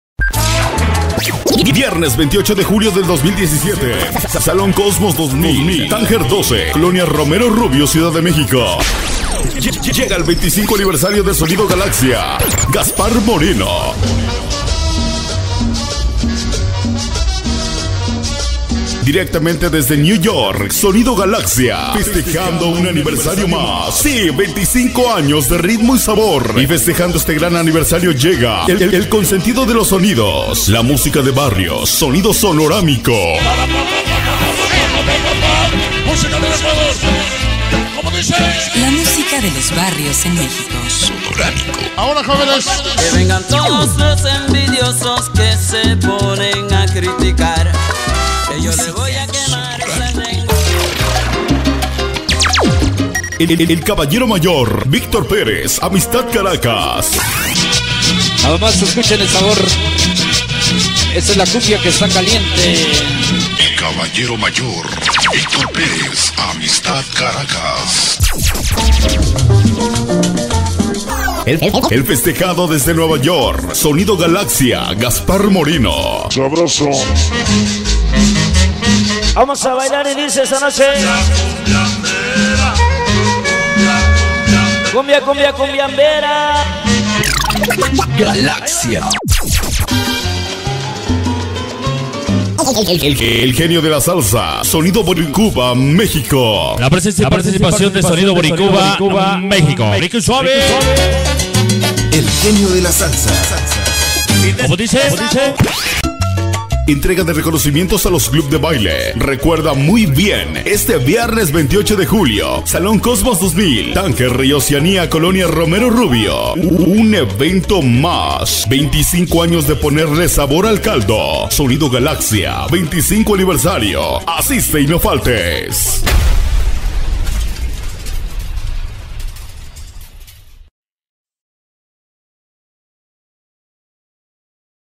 SONIDO GALAXIA NY. 25 ANIVERSARIO 2017
ESTE PROXIMO 28 de julio 2017
SALON COSMOS 2000 EN TANGER N12 Y OCEANIA COLONIA ROMERO RUBIO
DIRECTAMENTE DESDE NUEVA YORK
LLEGA EL 25 ANIVERSARIO DE SONIDO GALAXIA NEW YORK
GASPAR MORENO,FESTANDO 25 AÑOS DE PURO SABOR
CON LA PRESENCIA DE
SONIDO SONORAMICO--LA MUSICA DE LOS BARRIOS EN MEXICO
VICTOR PEREZ-EL CABALLERO MAYOR-AMISTAD CARACAS
EL FESTEJADO DESDE NY.SONIDO GALAXIA-GASPAR MORENO
EL GENIO DE LA SALSA -SONIDO BORINCUBA MEXICO
ENTREGA DE RECONOCIMIENTOS A CLUBS DE BAILE
SONIDO GALAXIA 25 ANIVERSARIO,ASISTE Y NO FALTES.
DISCOS BENJY INVITAAAAAAAAAAAAA.